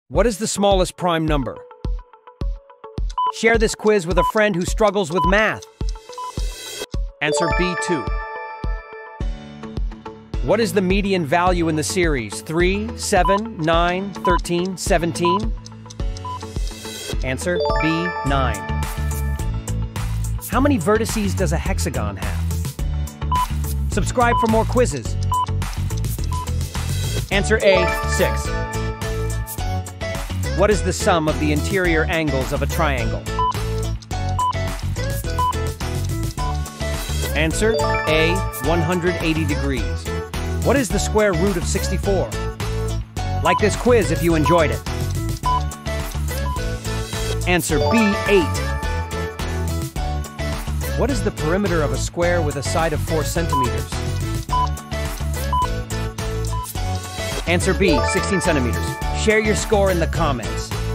Quiz Math: Geometry & Number Theory! 📐
Test your mathematical knowledge across geometry and number concepts in this comprehensive math quiz! 🔢 From prime numbers to geometric properties, challenge your understanding of fundamental mathematical principles! ⚛️